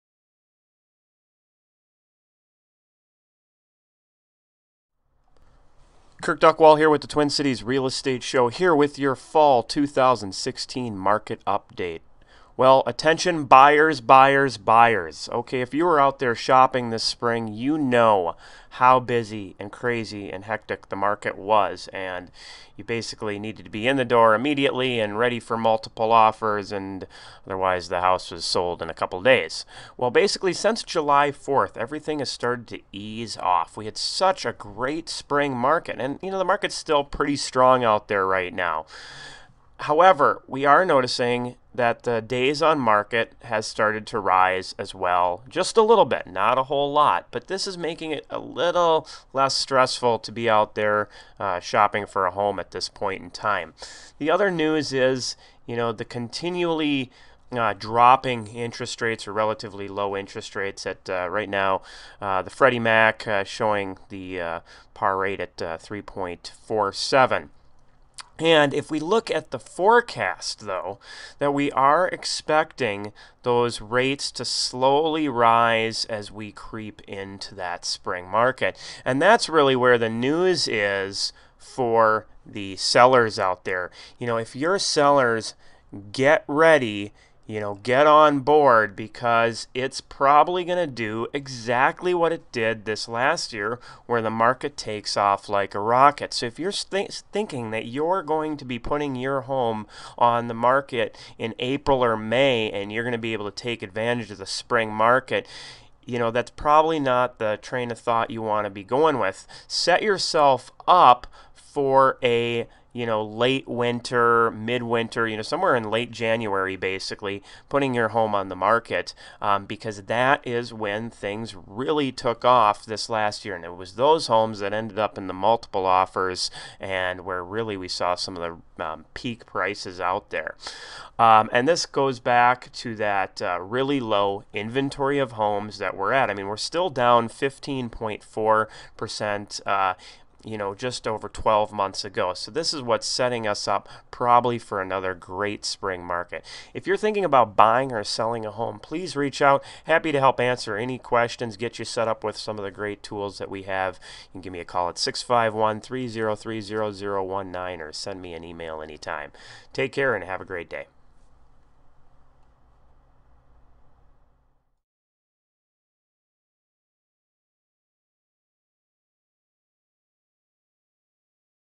Twin Cities Real Estate Market Update Fall 2016
Real estate market stats for the Minneapolis and St. Paul area